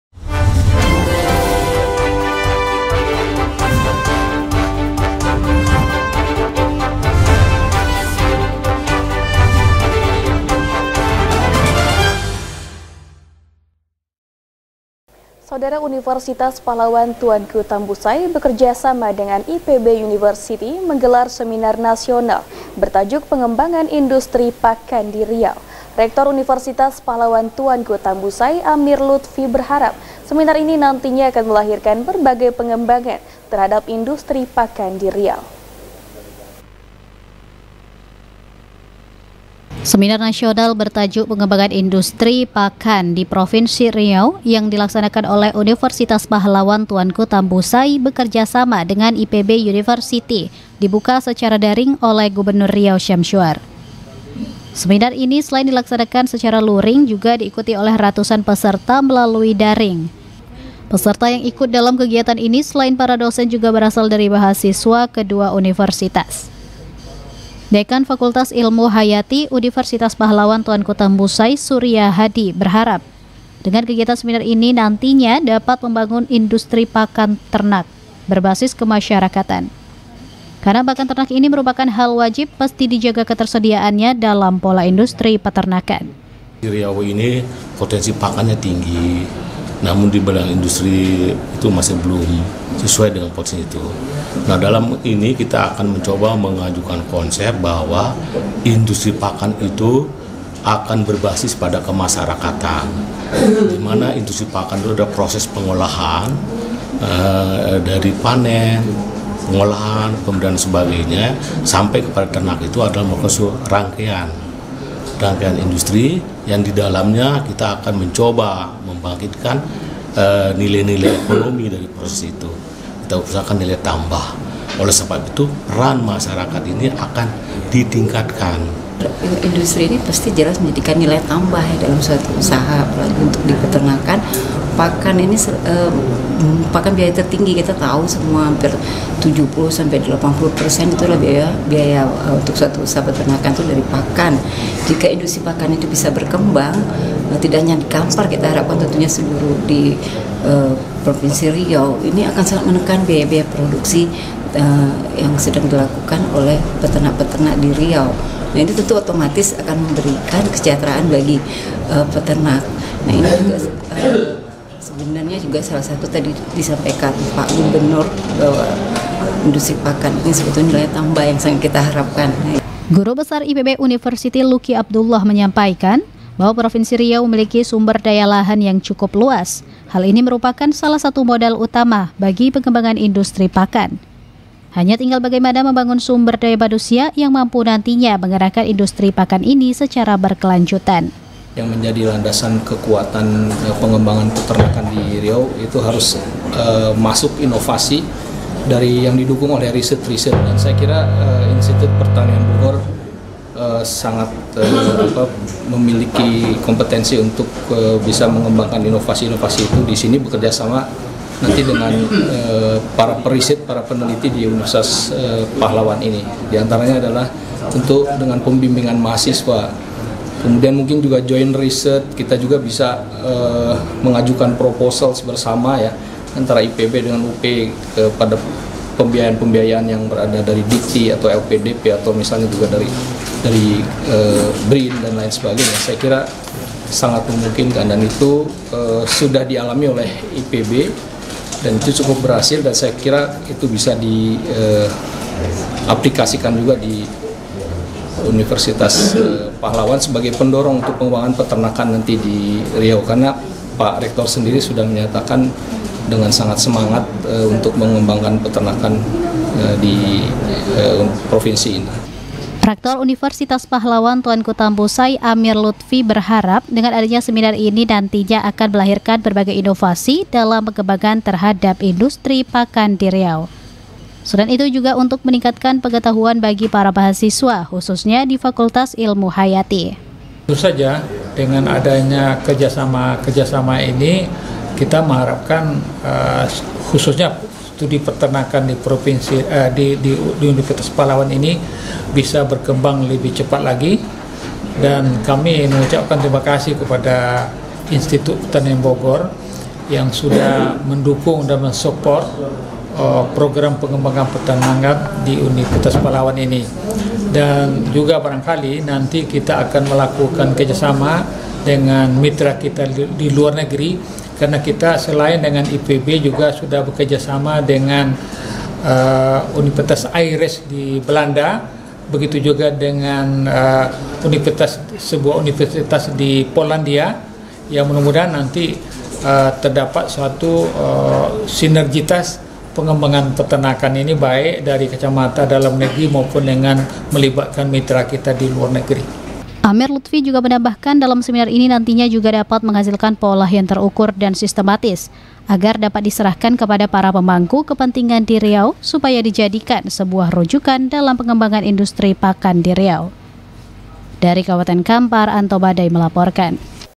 UNIVERSITAS PAHLAWAN TUANKU TAMBUSAI BERSAMA IPB GELAR SEMINAR NASIONAL
Universitas Pahlawan Tuanku Tambusai, bekerja sama dengan IPB University, menggelar Seminar Nasional, bertajuk pengembangan Industri Pakan di Riau. Rektor Universitas Pahlawan Tuanku Tambusai, Amir Lutfi berharap , Seminar ini nantinya akan melahirkan berbagai pengembahan, terhadap Industri Pakan di Riau.